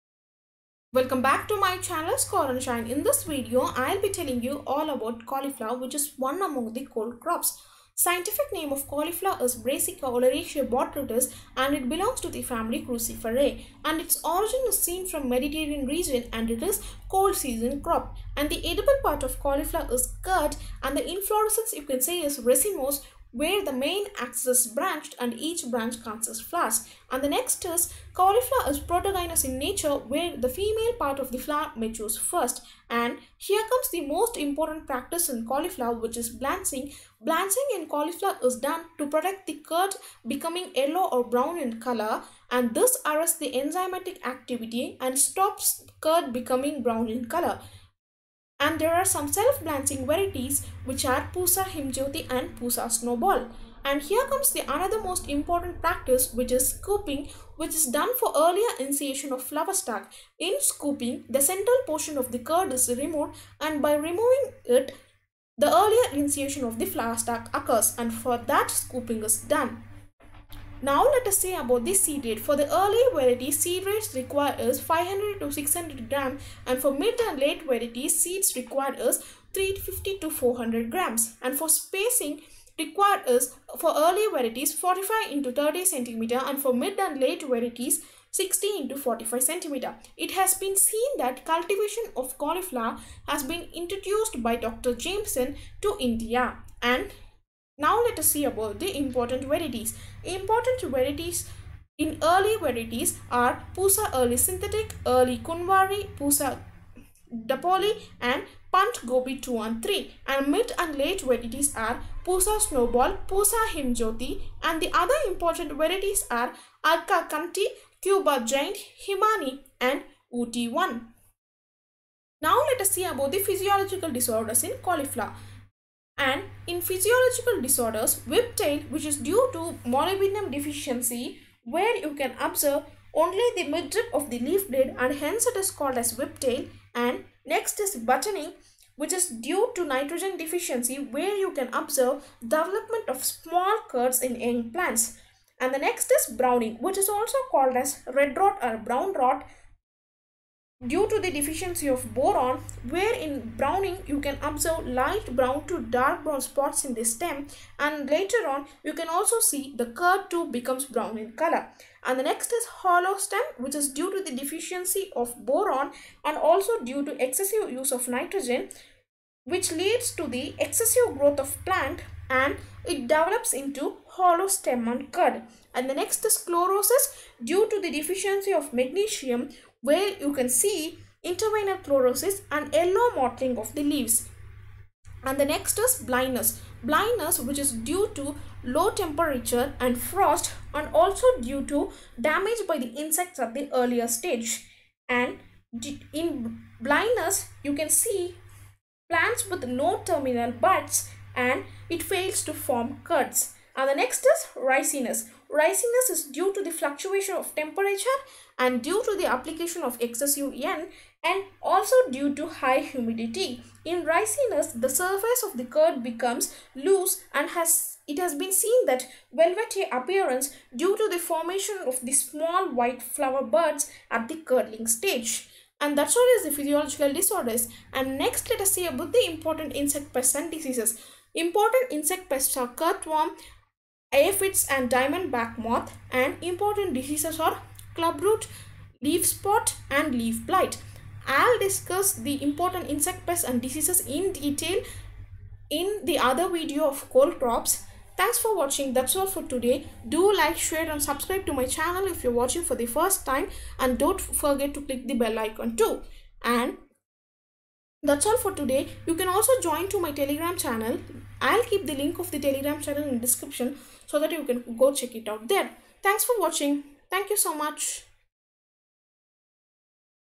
CAULIFLOWER | ALL ABOUT CAULIFLOWER | IBPS SO | RRB SO | JRF | ARS | IFFCO | AGRI. COMPETETIVE EXAMS
THIS VIDEO IS ALL ABOUT CAULIFLOWER. CONTENT IS ACCORDING TO THE EXAM POINT OF VIEW.

HOPE THIS VIDEO WILL BE USEFUL FOR MANY SELF PREPARING STUDENTS TO CRACK AGRI COMPETETIVE EXAMS.

ALSO JOIN TO THE  TELEGRAM CHANNEL

STAY SAFE.